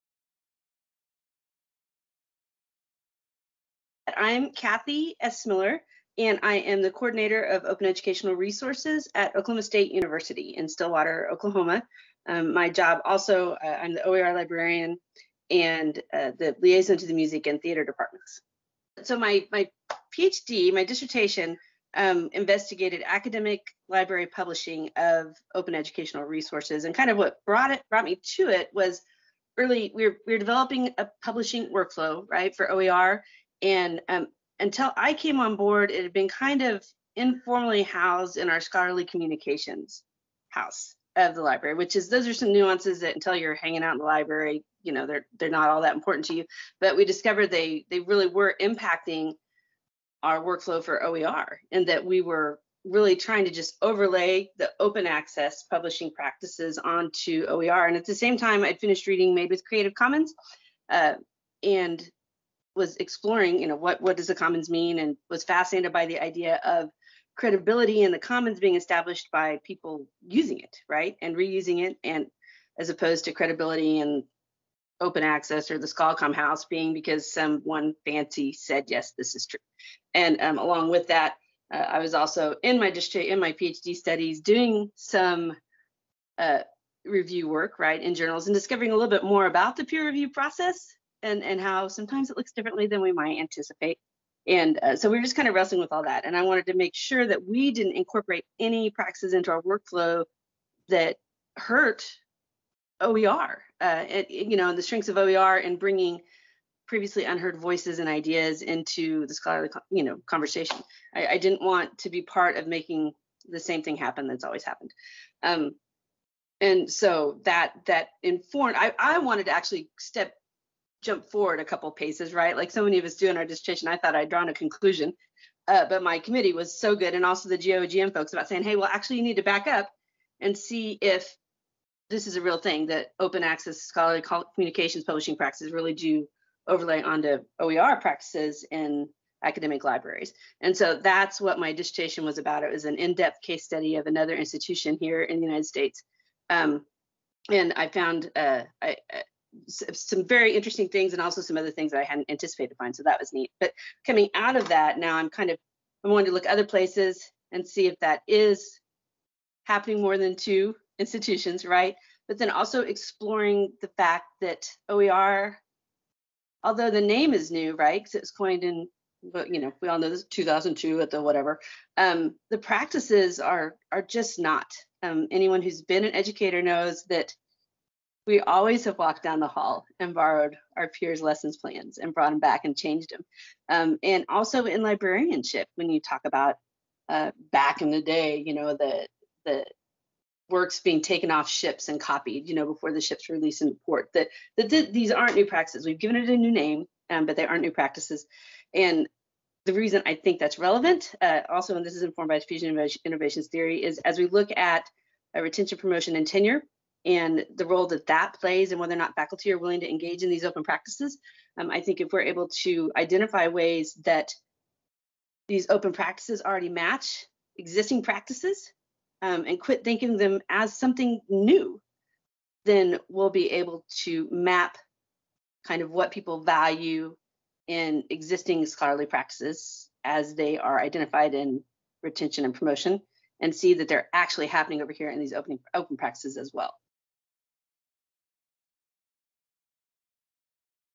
Kathy Essmiller: Research Profile (September 2021)
GO-GN member Kathy Essmiller (Oklahoma State University) on the focus of her doctoral research.

This interview was recorded on 20 September 2021.